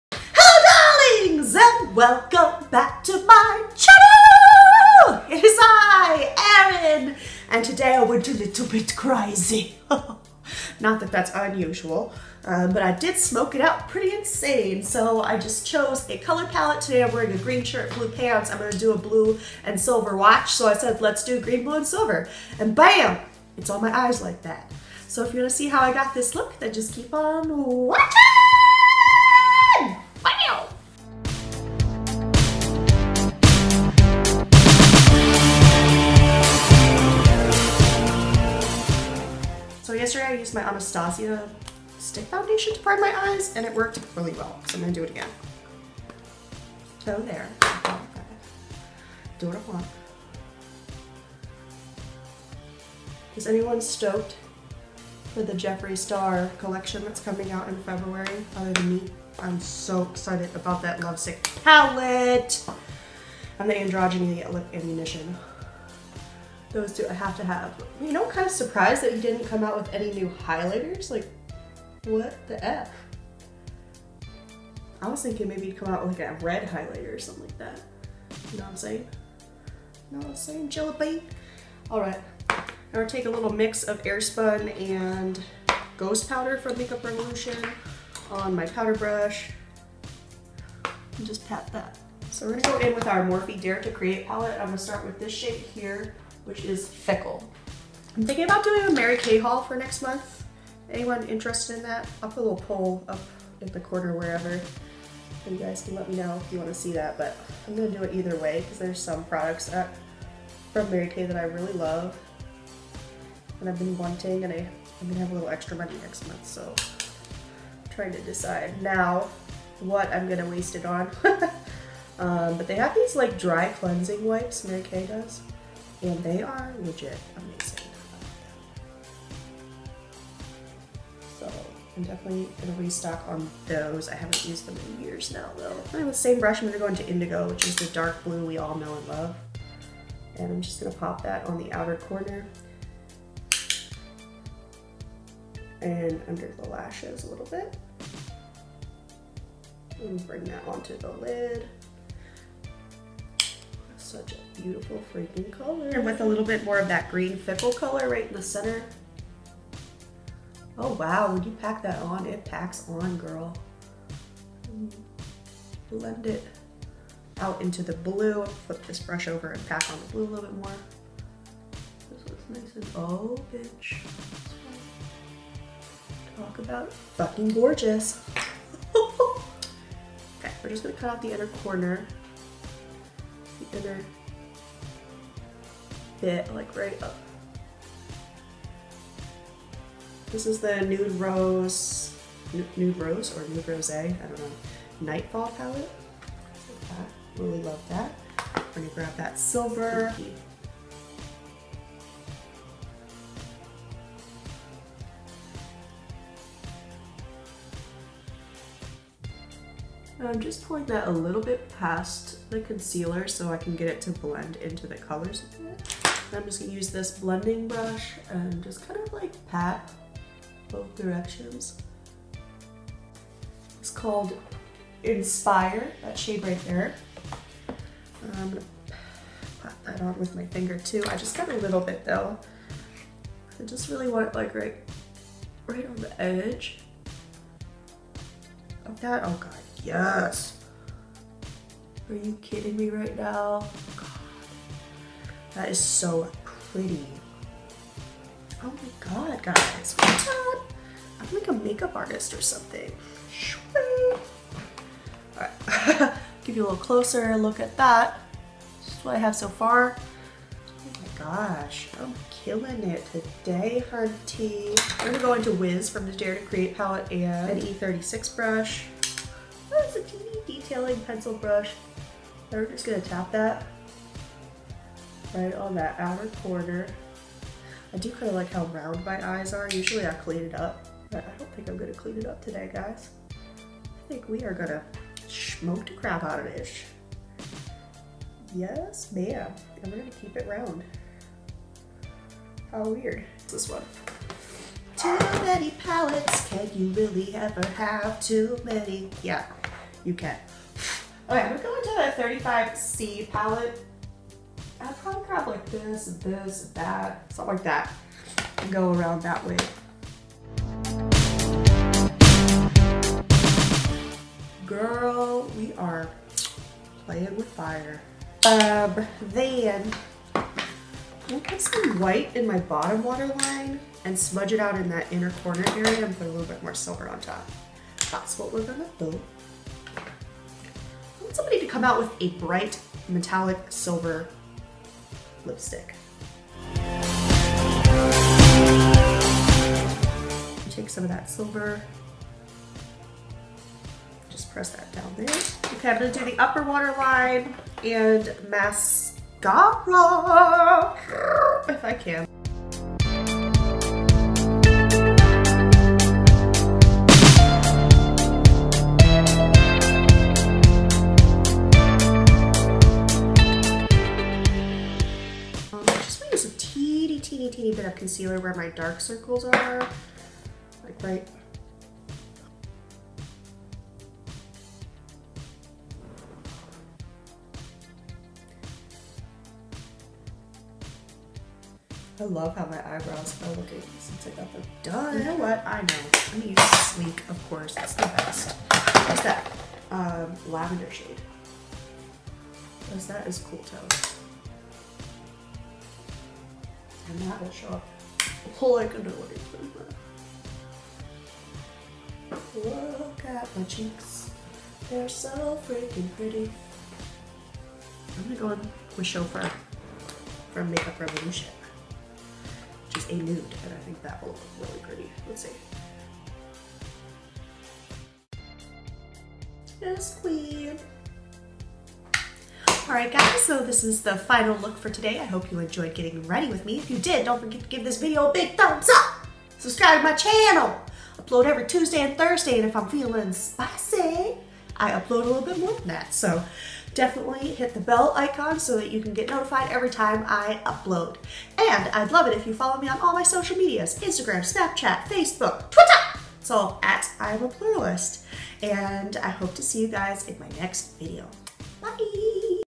Green smoke | eotd | grwm
Usually when I’m working I don’t wear any foundation at all because I sweat it off in a matter of minutes. But I like to wear eye makeup anyway! So here’s a tut of just the eyes. 👀👀
Yesterday's massively blown out green and silver smokey eye! Green and blue from @morphebrushes 39a palette (indigo and fickle) silver from @bhcosmetics night fall palette .
Mascara is my fave monsieur big mascara @lancomeofficial .
Liner is @nyxcosmetics retractable eyeliner .